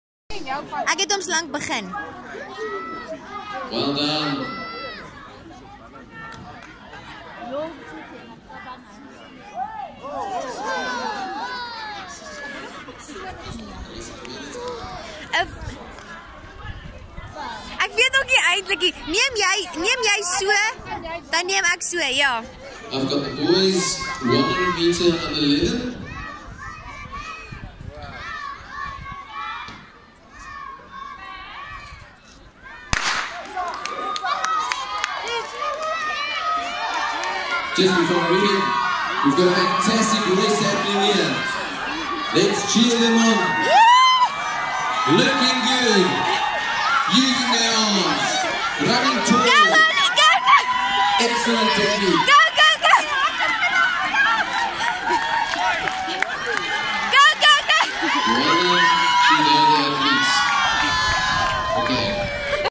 Downsindroom Atletiekdag
“Oliver bring eindelose vreugde. Mense ken amper nie my naam nie, ek is net Oliver se pa. Hy het ’n manier om in mense se harte in te kruip. Ouers en kinders s’n. En sy hart is so sag. Hy vergewe altyd maklik. Nie soos party mense wat vir dae dikmond rondloop nie.”